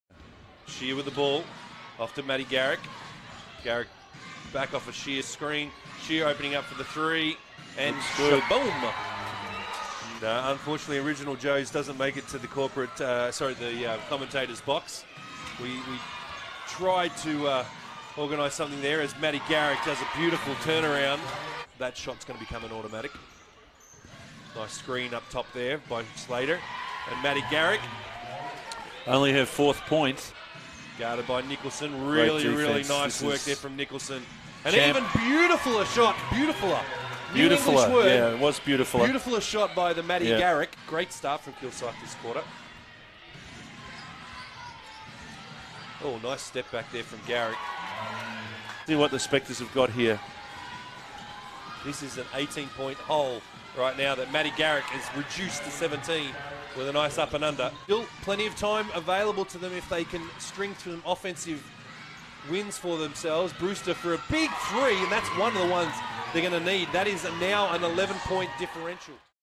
Maddie Garrick Posts 11 points & 11 rebounds vs. Kilsyth
Maddie Garrick Posts 11 points & 11 rebounds vs. Kilsyth, 08/10/2019